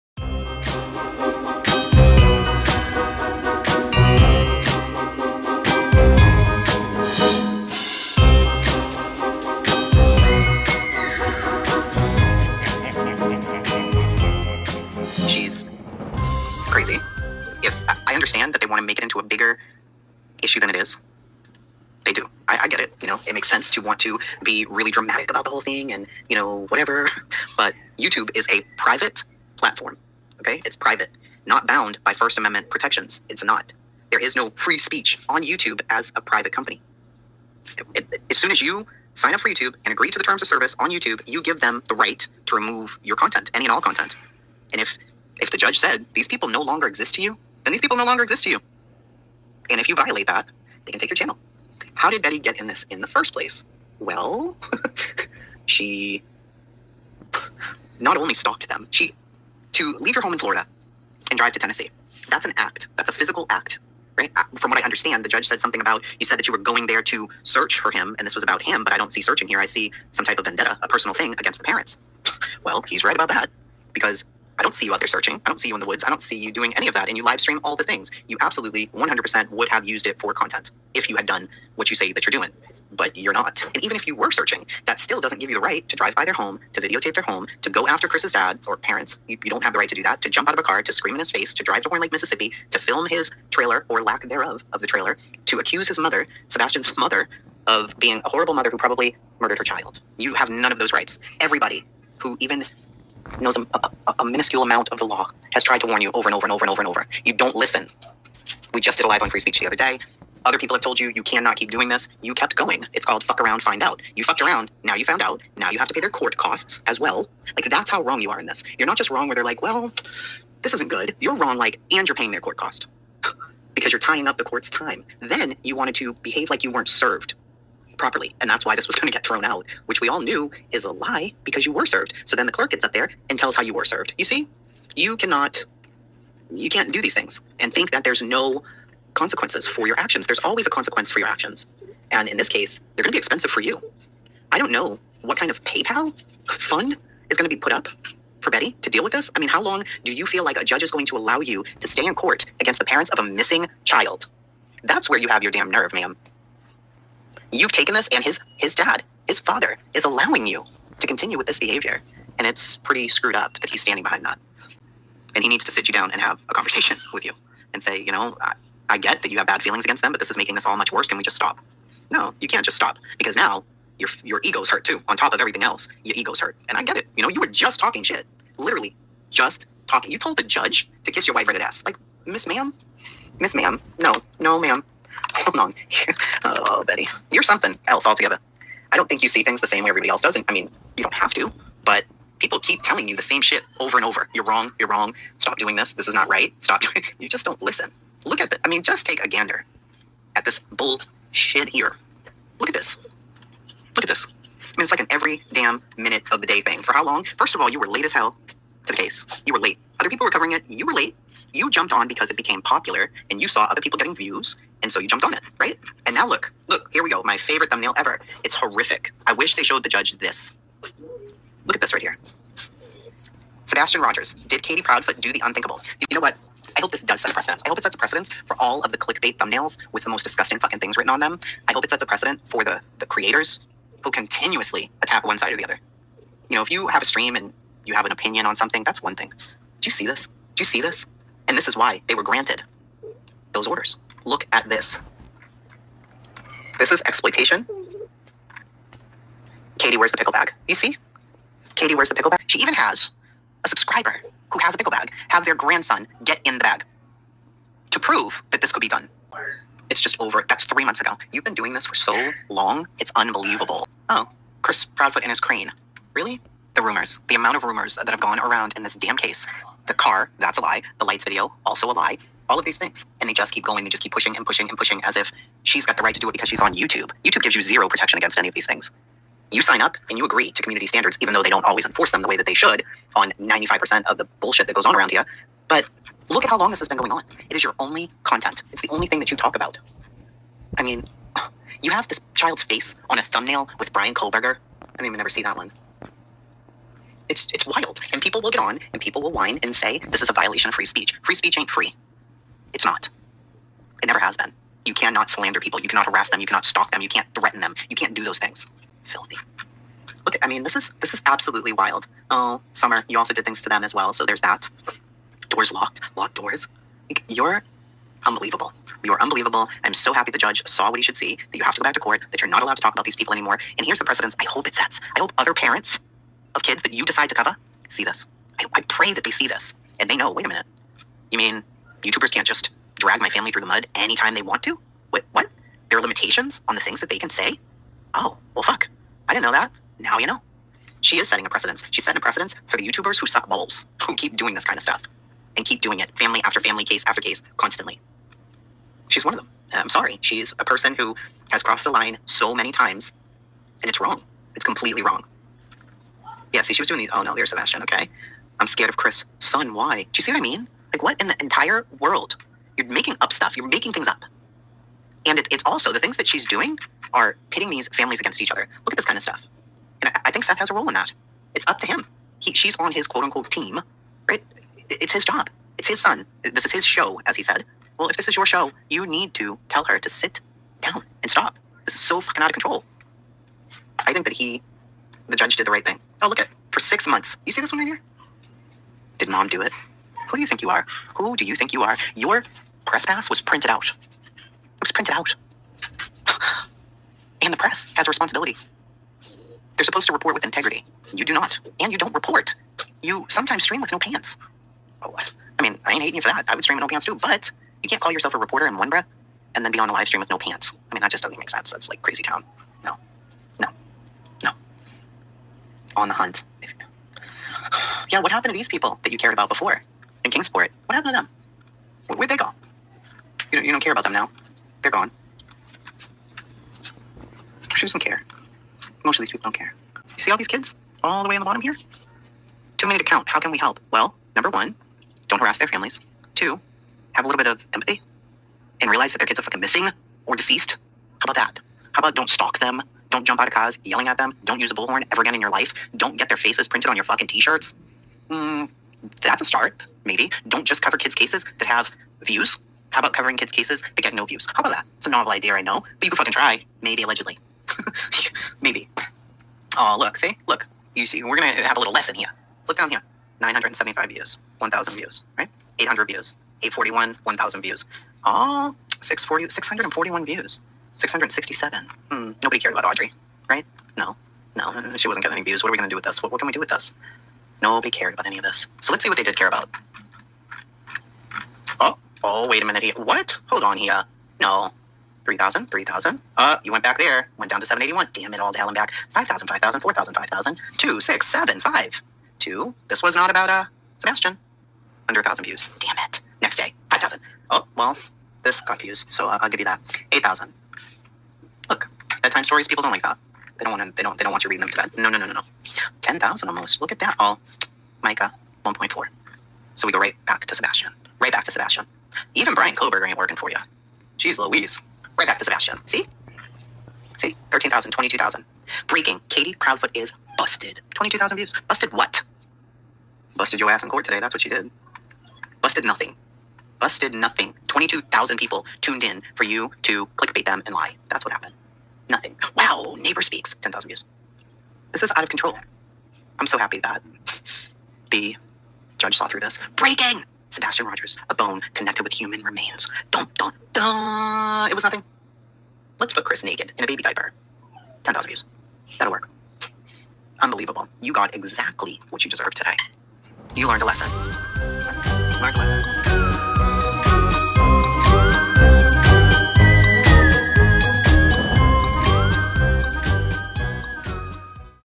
has a message for @BULLHORNBETTY
The judge ruling in the case between stalker and harasser, Bullhorn Betty, and missing teen Sebastian Rogers' mother and stepfather, is showing that this type of behavior, will NOT be tolerated. Chasing Truth speaks on it...